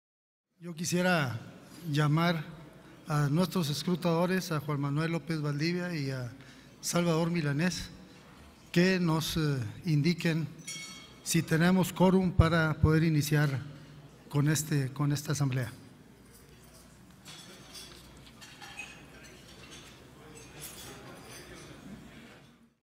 Enero 2015 Momento 3: "Asamblea General de Asociados 2015" - Convocatoria
Con fundamento en los artículos 12, 13, 15, 16, 17, 18 y 19 de los estatutos de la Confederación USEM, A.C. se emitió la CONVOCATORIA a la Asamblea General de Asociados 2015 que se llevó a cabo en la Ciudad de México D.F. Nombramiento de Escrutadores por el Ing. Lázaro Tamez, Presidente de Confederación USEM.